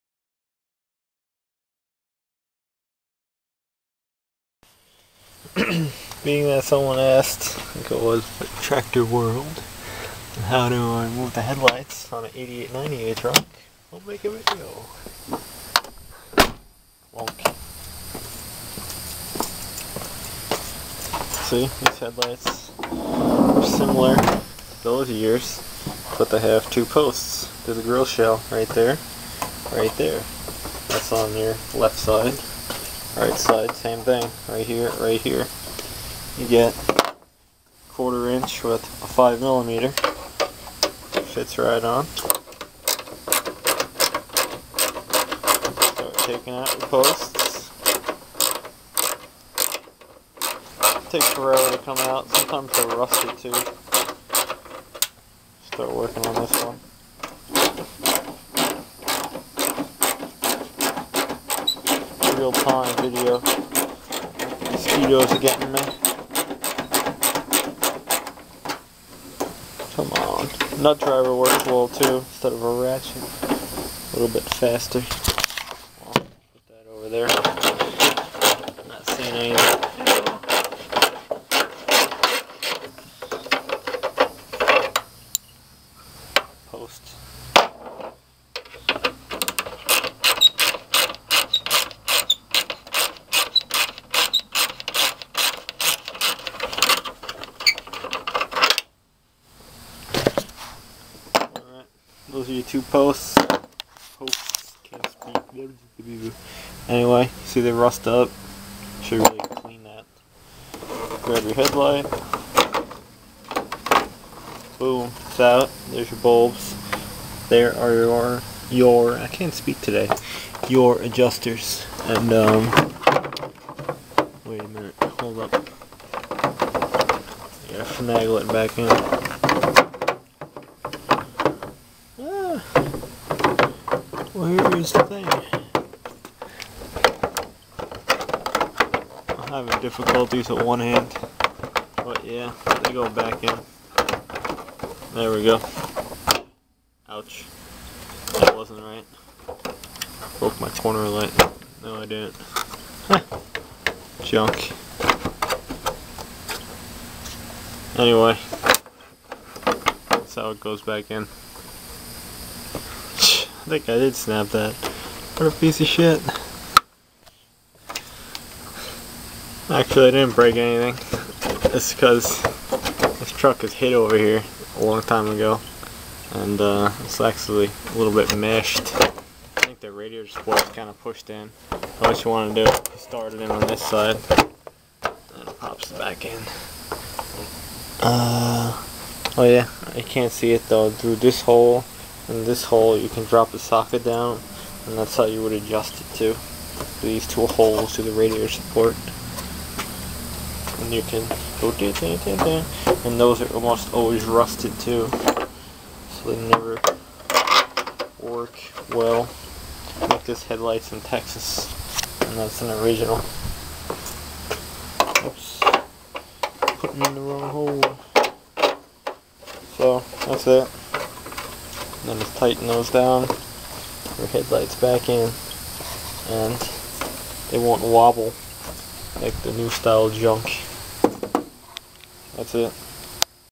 88- 98 Fullsize Chevy GMC headlight removal.
Heat makes the plastic stick together too, further sticking in the lights